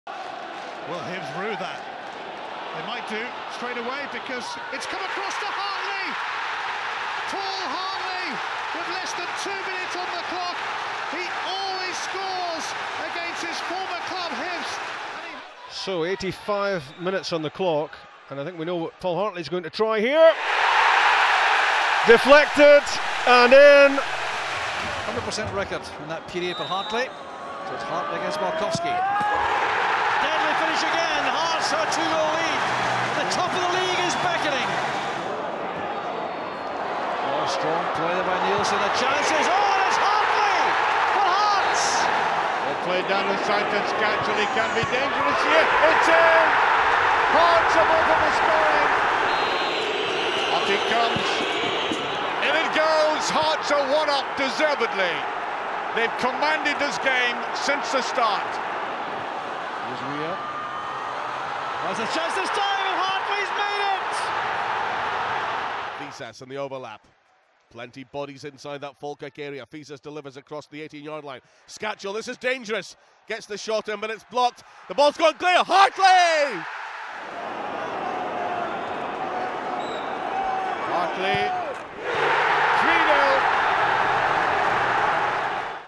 Scottish Football Hotshots - Paul Hartley
Paul Hartley was one of the best goal scoring midfielders in the Scottish top flight whilst playing for Hearts in the mid-2000s. Check out some of his best moments here!